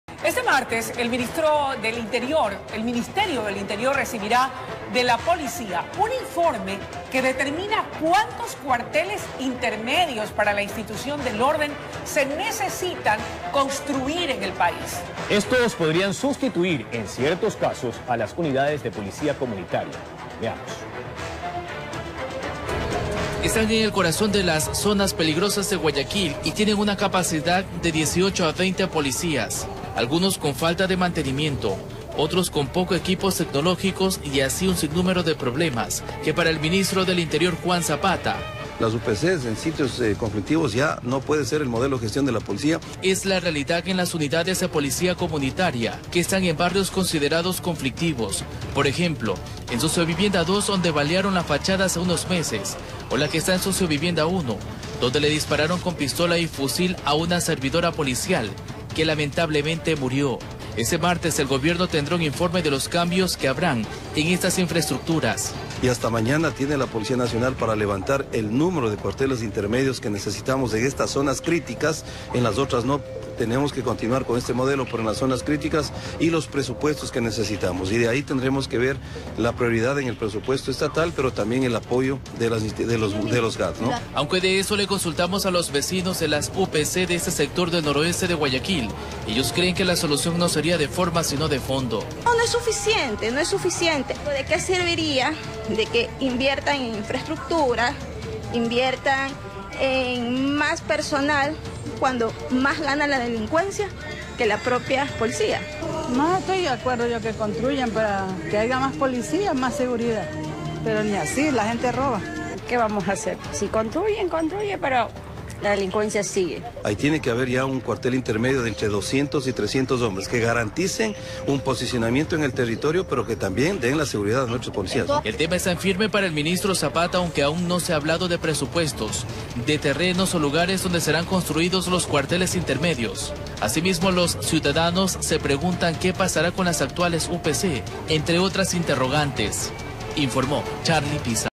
Anuncian construcción de cuartel intermedios
El ministro del Interior, Juan Zapata, ante el incremento de violencia e inseguridad nacional indicó que el gobierno realizará un informe para la construcción de cuarteles intermedios. El objetivo es construir cuarteles intermedios en las zonas conflictivas. Los cuarteles intermedios reemplazarían a las UPC.